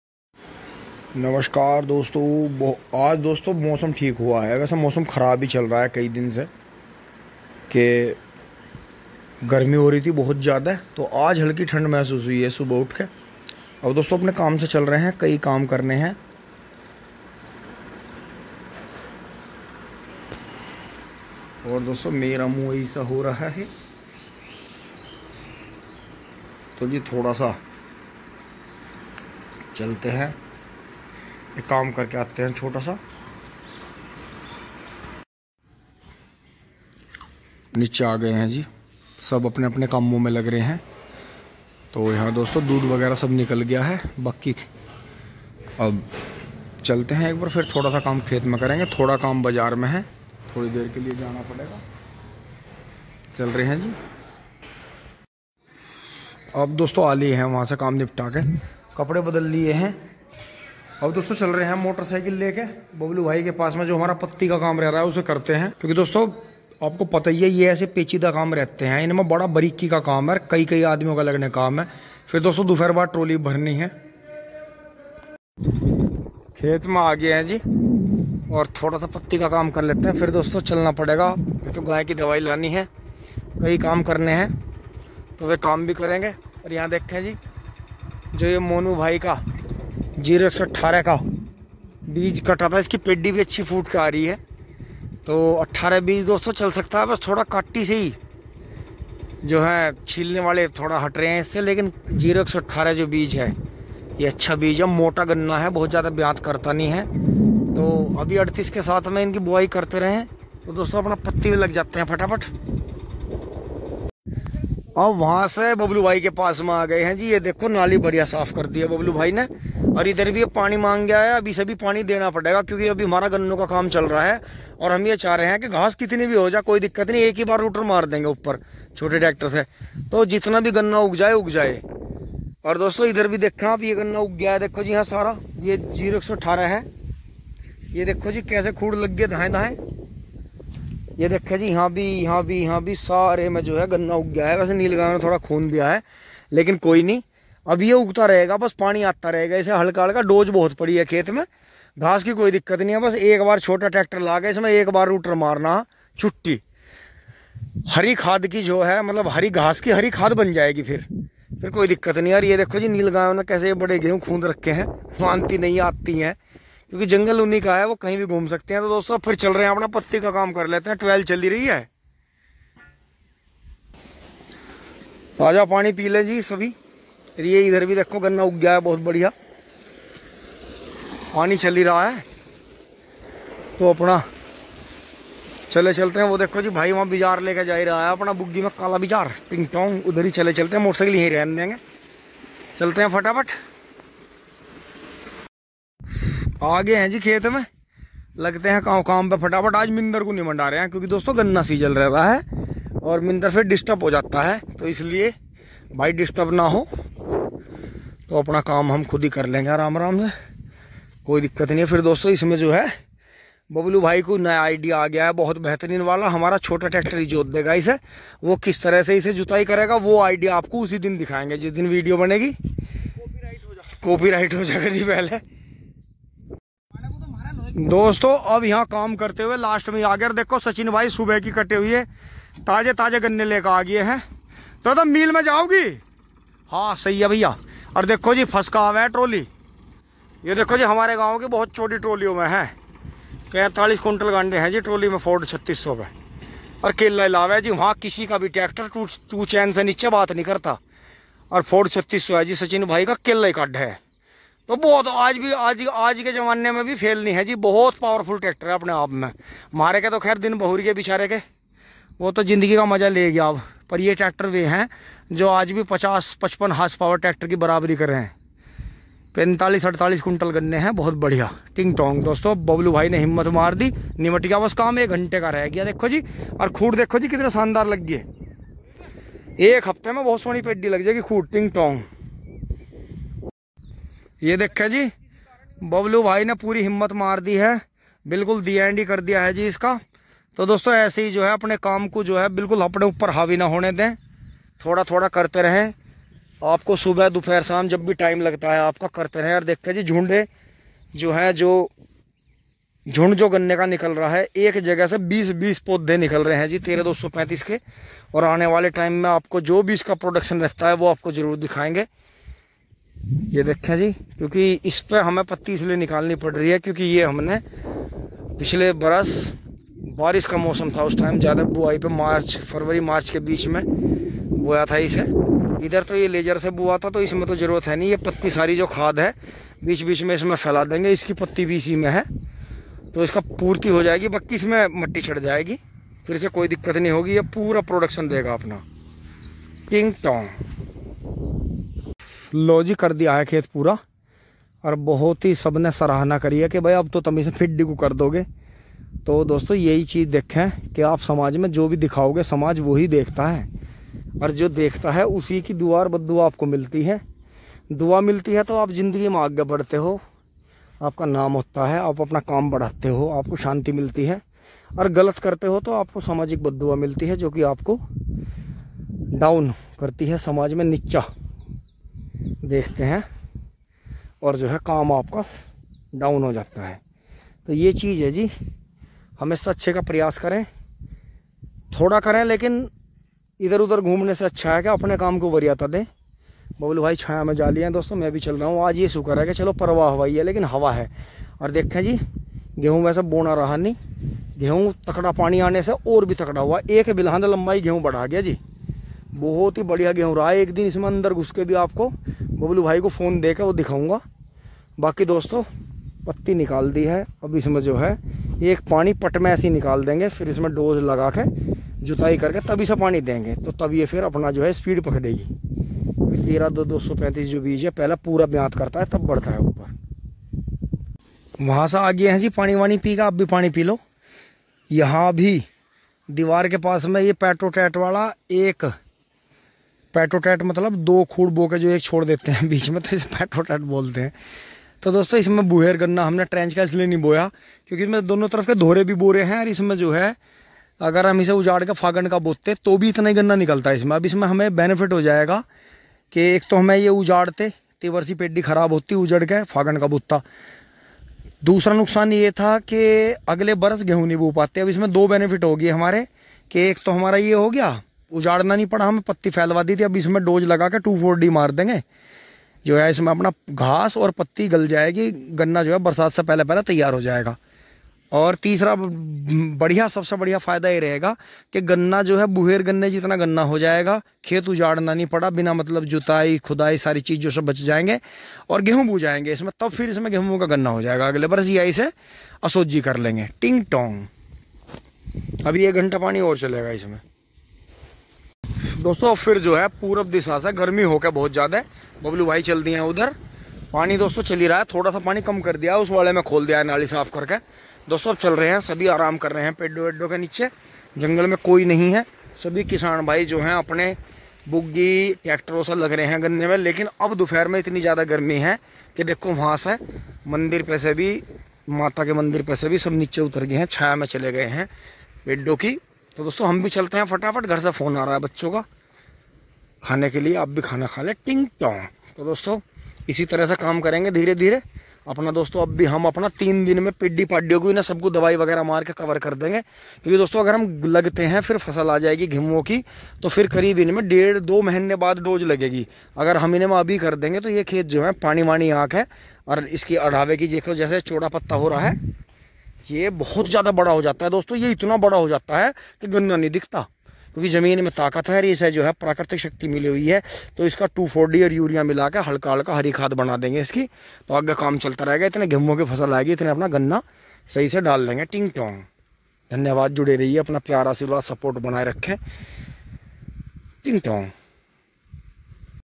क्या 13235 गन्ने की पैड़ी हो जाएगी..सूखी पत्ती का काम फाइनल कर दिया..Do your best
Puneet Punia channel vlogs..Pabarsa Daurala Meerut UP..धन्यवाद..जुड़े रहिए 🙏🎖❣️🎋🚜🐃🐂🏭